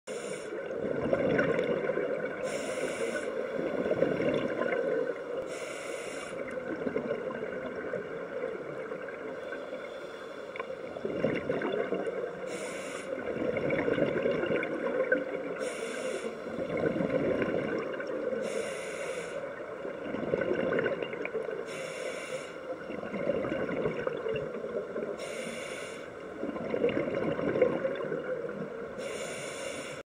Spanish Lobster in Turks and Caicos with Ruth and Jackie
Spanish lobster swimming while scuba diving